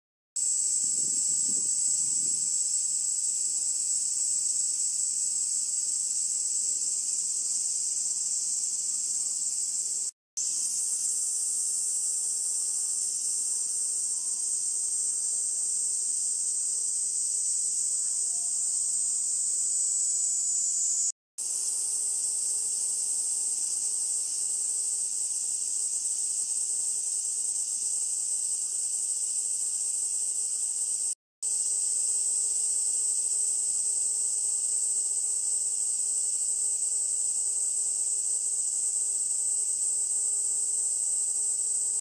【ニイニイゼミ】夏の訪れを感じる蝉（音あり） 【4K】
ニイニイゼミは、梅雨の最中の6月下旬から7月頃にかけて成虫になり、他のセミより早く鳴き始めます。サクラの木によく集まり、人の手が届くような低い枝にも止まります。

見沼田んぼ周辺、特に見沼代用水沿いにはたくさんのサクラの木がある関係でニイニイゼミはよく見かけます。